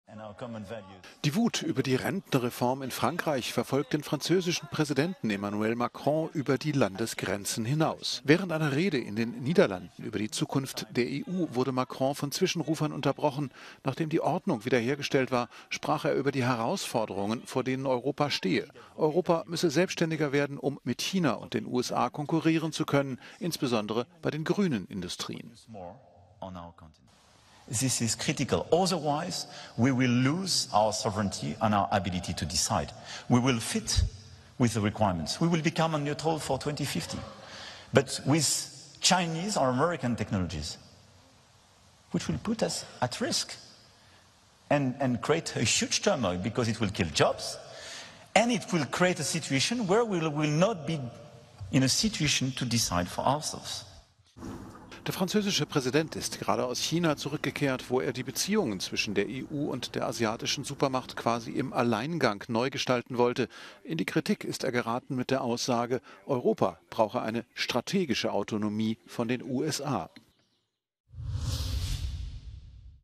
"Präsident der Gewalt und der Heuchelei" - Protest gegen Macron in den Niederlanden
Die Wut über die Rentenreform in Frankreich verfolgt den französischen Präsidenten Emmanuel Macron über die Landesgrenzen hinaus. Während einer Rede in den Niederlanden über die Zukunft der EU wurde Macron von Zwischenrufern unterbrochen.